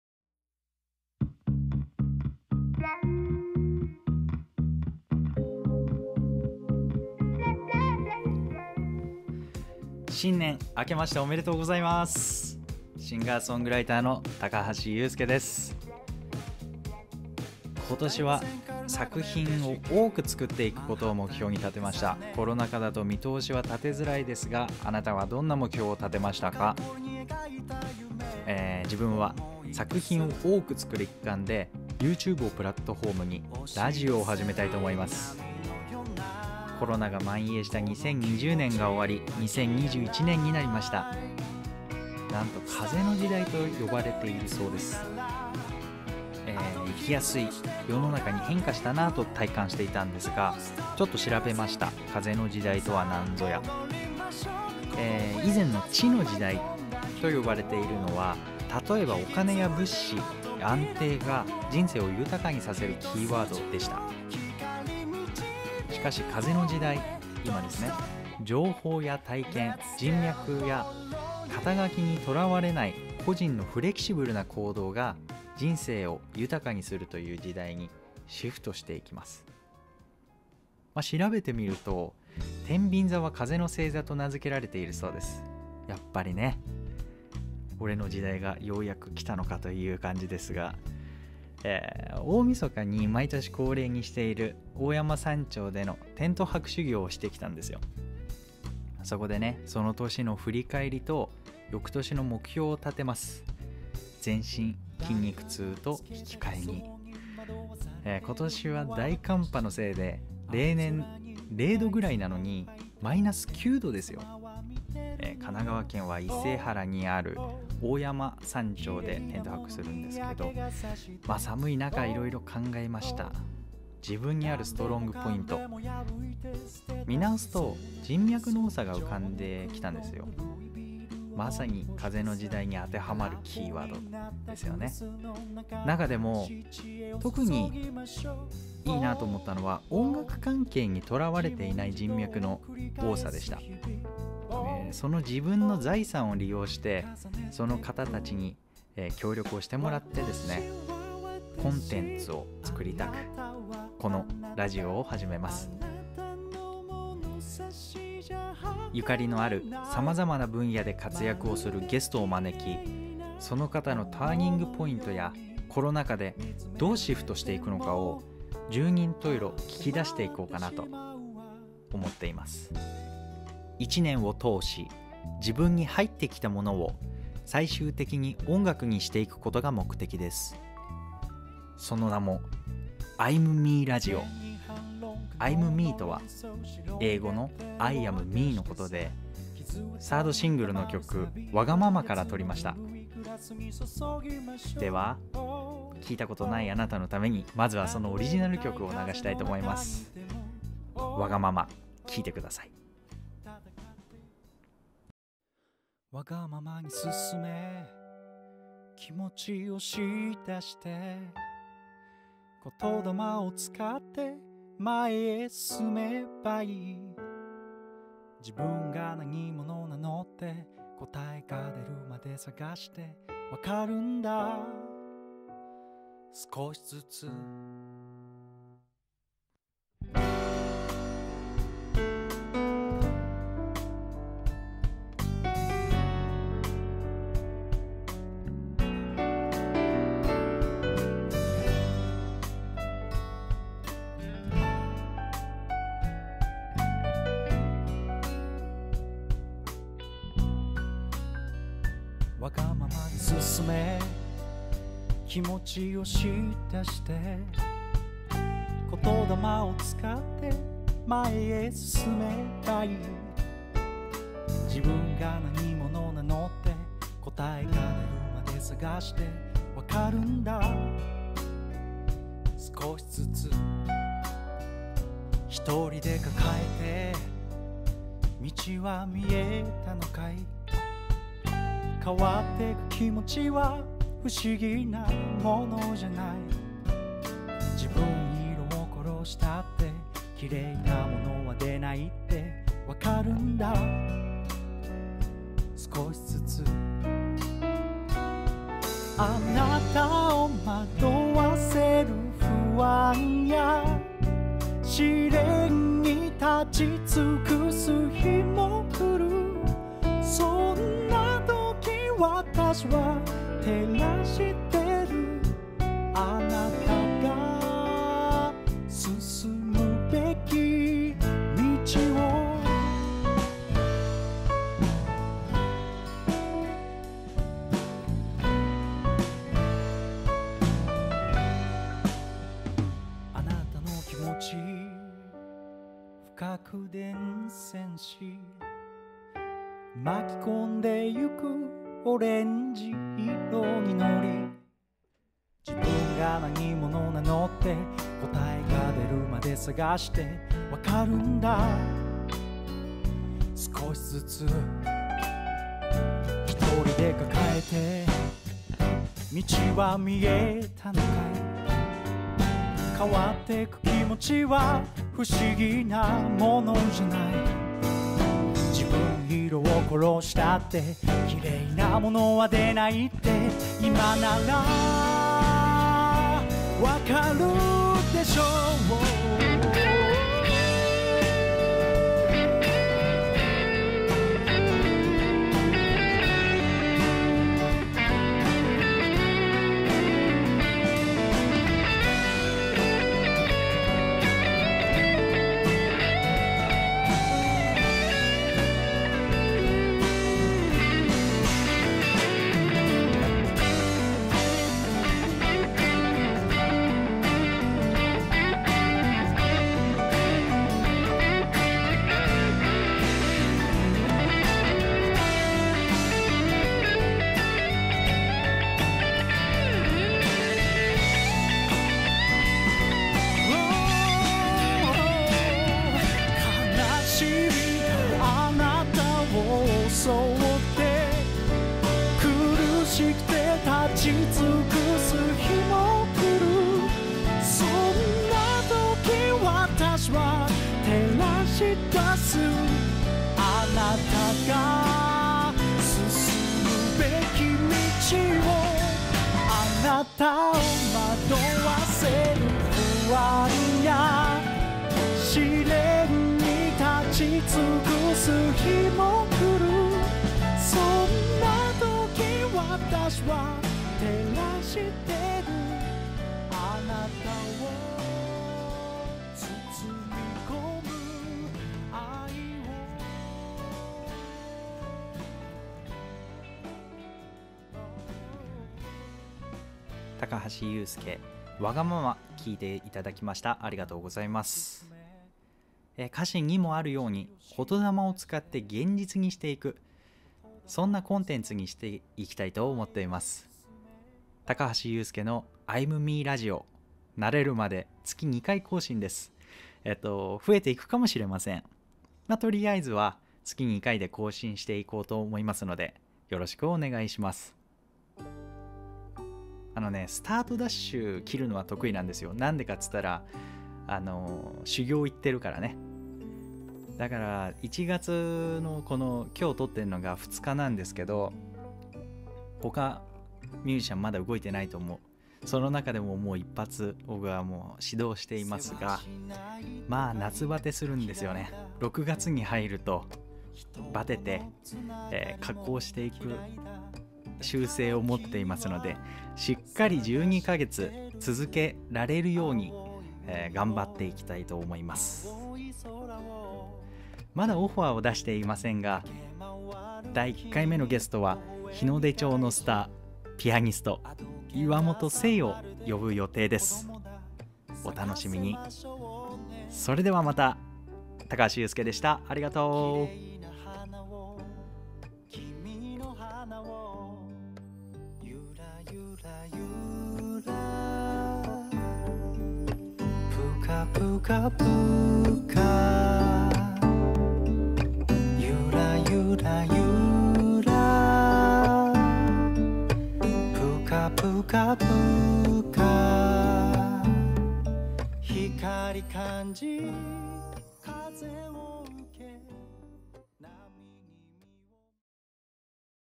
アイムミーラジオ Vol.０
風に運ばれるが如く歌を届けるシンガーソングライターによるラジオ番組
「我がまま」に「自分らしく生きる」をテーマにゲストと対話する企画。